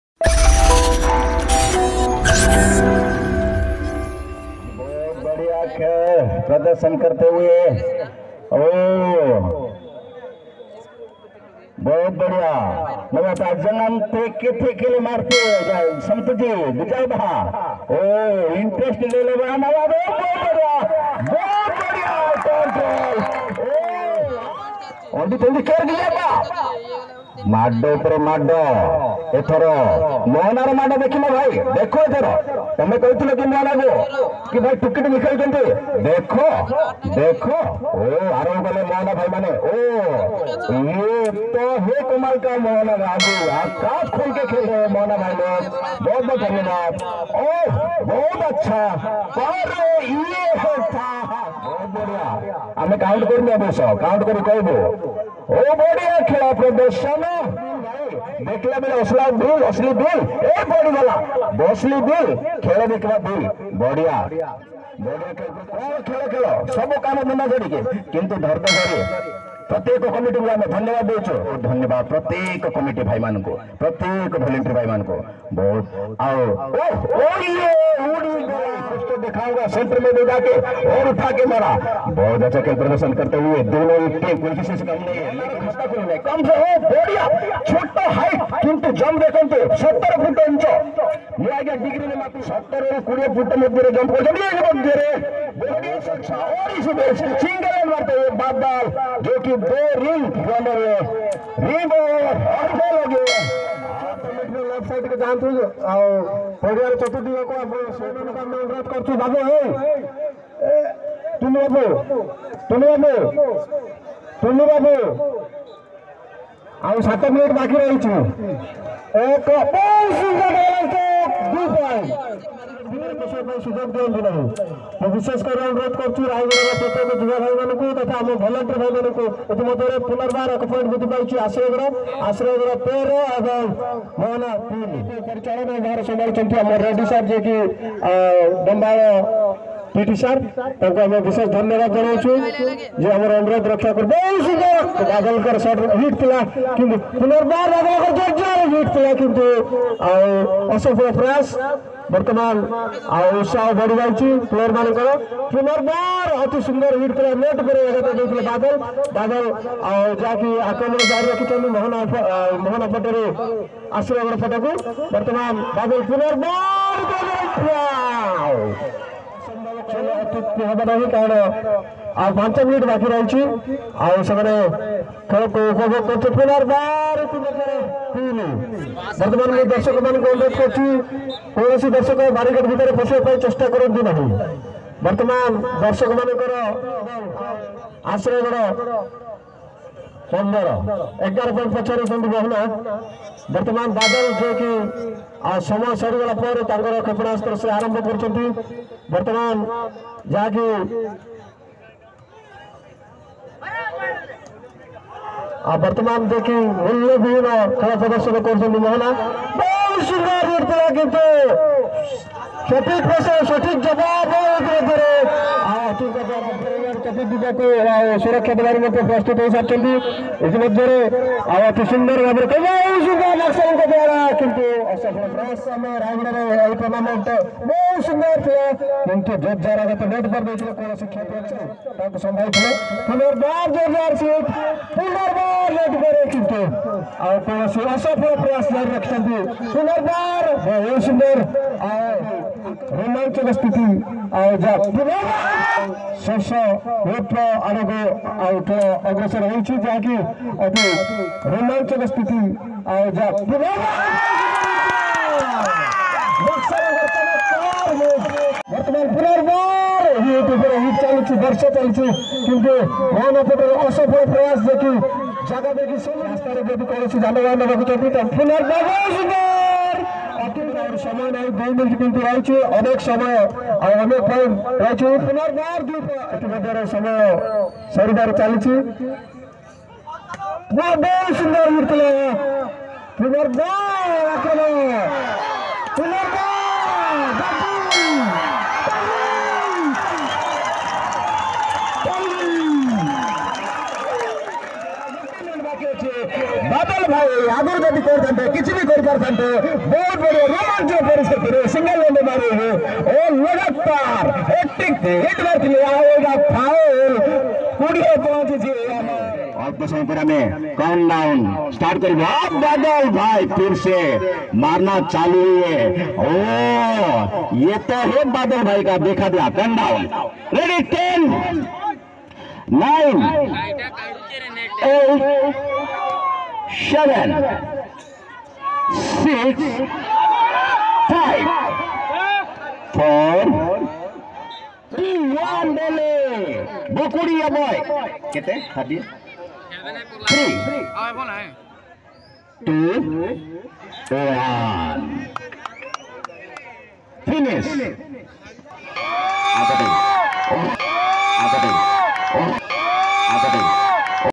Mohona vs Ashrayagada | ମୋହନାର ଚମତ୍କାର ଖେଳ ସତ୍ୱେ ପରାଜୟ | Sunam vs Badal | Final Match at Rayagada
Dhaba Volleyball Tournament
Trailer Video
Final Match at Rayagada, Gajapati
ମୋହନାର ଚମତ୍କାର ଖେଳ ସତ୍ୱେ ପରାଜୟ
Sunam vs Badal
Ashrayagada vs Mohona
if Full Match Video Need then Comment Bellow

Team Emmanuel Ashrayagada
Sunam, Ishak, Naxal, Rupa, Sampurna, Monoj, Binu and etc.

Team Mohona
Badal, Atu, Monoj, Rahul and etc.